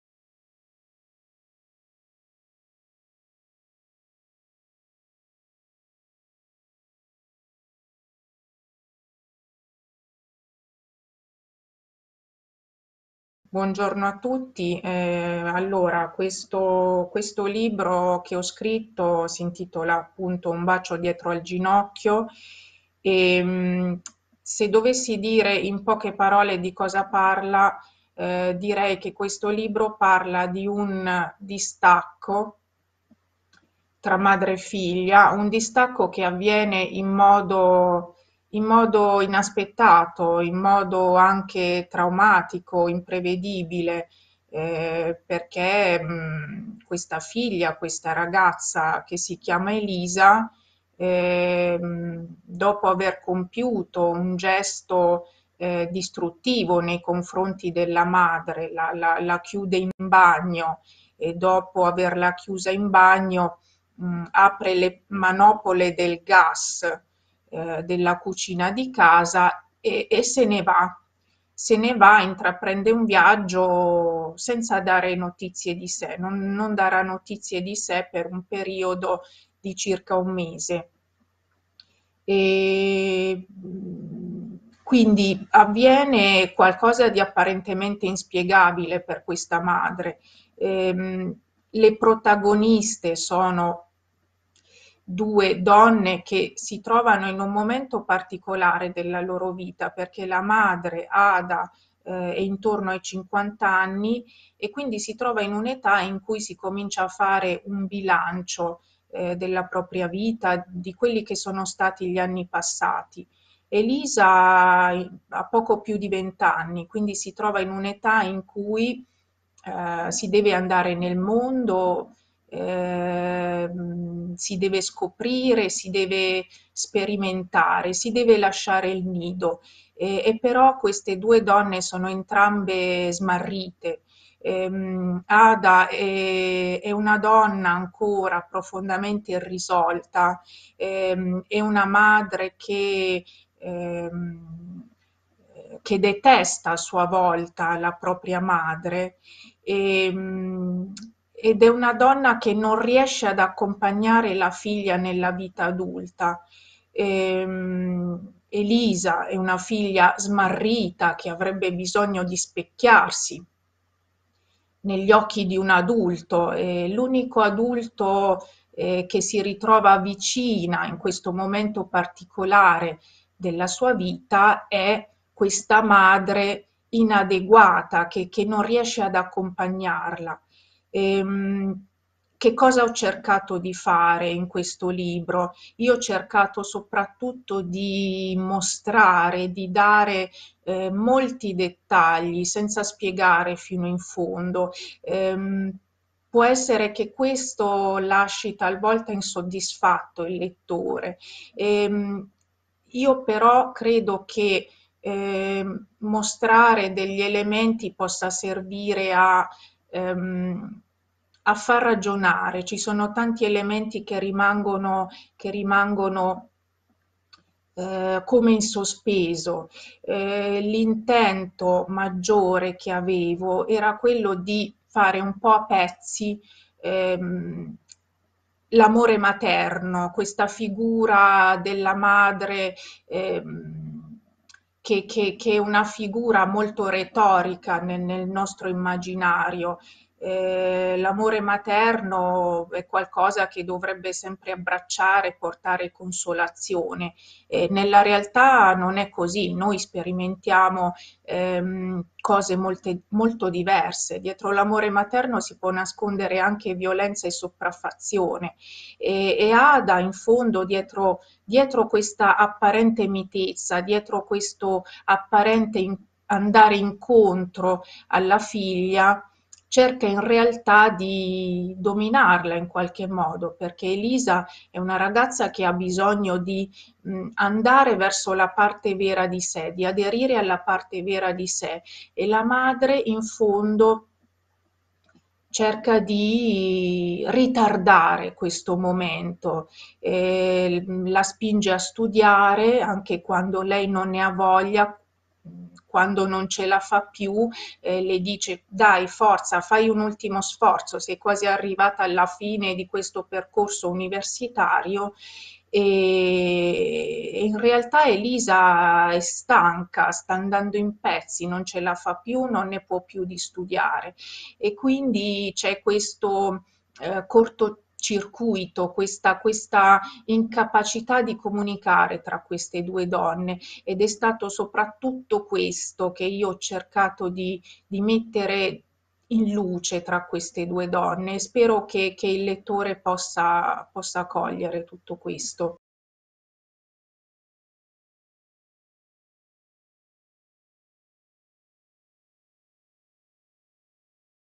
Premio The Bridge 2021: Carmen Totaro, "Un bacio dietro le ginocchia" - Stanze italiane
Candidata al Premio The Bridge 2021:
Carmen Totaro, "Un bacio dietro le ginocchia", Einaudi

Una madre, una figlia poco più che ventenne, le quattro pareti della loro casa e un duello di parole e silenzi. Poi una mano chiude a chiave una porta, e quel gesto è uno spartiacque tra il prima e il dopo. Ci sono libri che ti trascinano nel cuore di un mistero rispettandolo, facendolo risuonare sino all'ultimo. Questo libro è così: un ingranaggio narrativo perfetto. Ti catapulta con forza nella testa di due personaggi feriti e vivissimi, ti fa vedere con i loro occhi, sentire con la loro pelle, procedere a tentoni con loro. A un passo dal mistero.